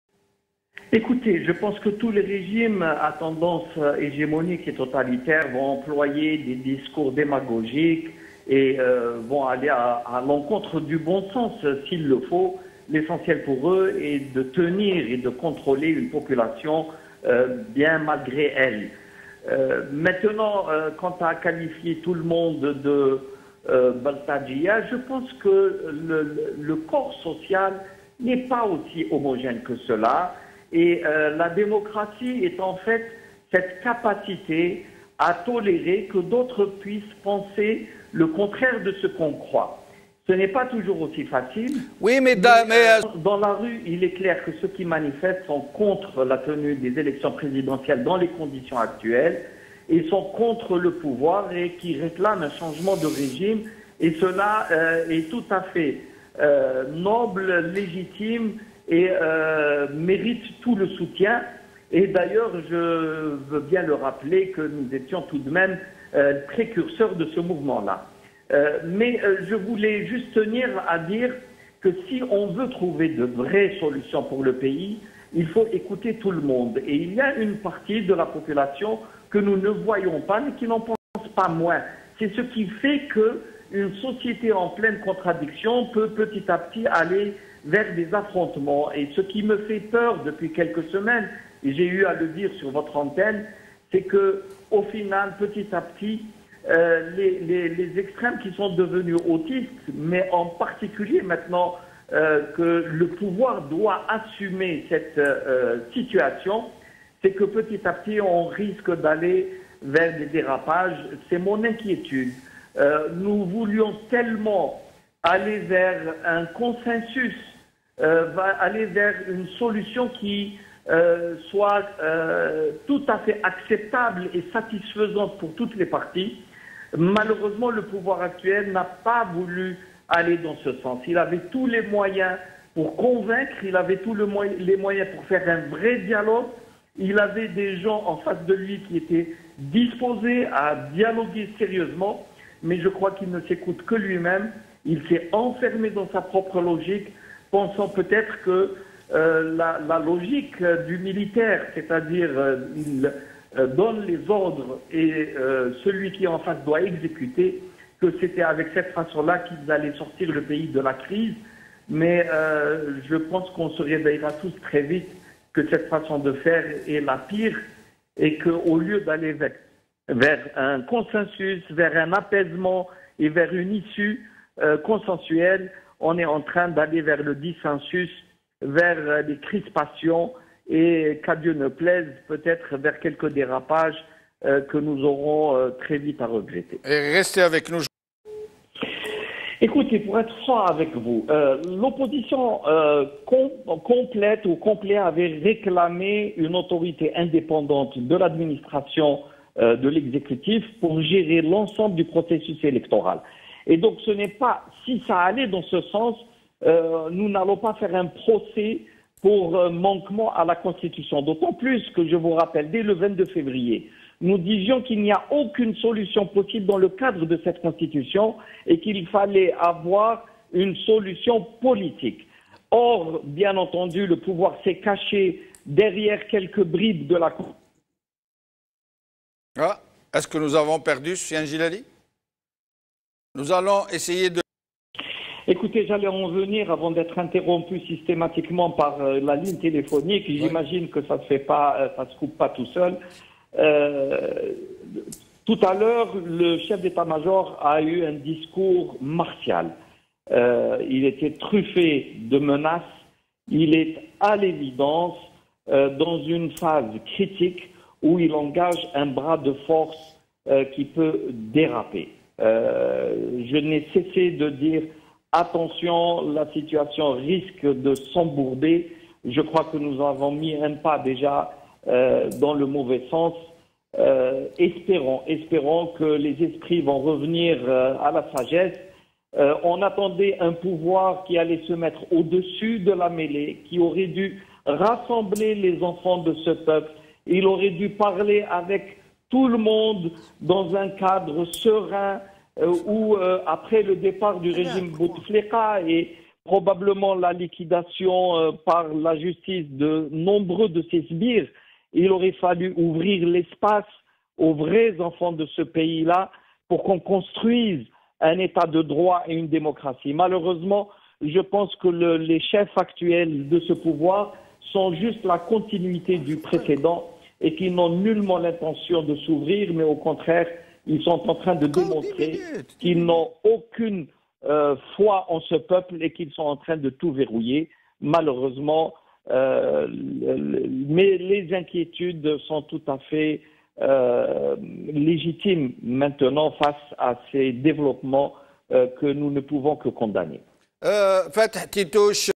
Une personnalité politique aux arrêts est la condamnation de la démocratie.
Un Etat de droit respecte la liberté de parole. Une personnalité politique aux arrêts est la condamnation de la démocratie. Mon soutien à Karim Tabou